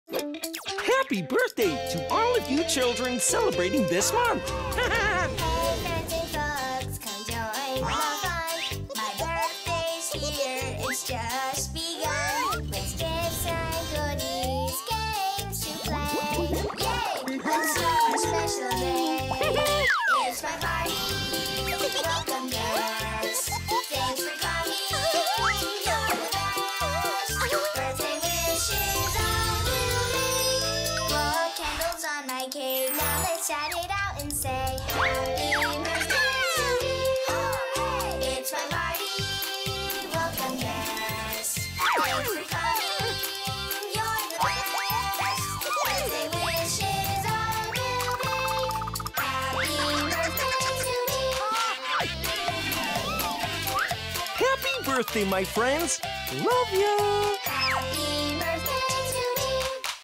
BabyFirst BDay - December BDay - 32 - Isabella , Kai , Milania, Shannon Jr., Juliana, Nathaniel ,
BabyFirst wishes all the babies in December a very happy birthday filled with giggles, smiles and hugs. BabyFirst. Watch your baby blossom.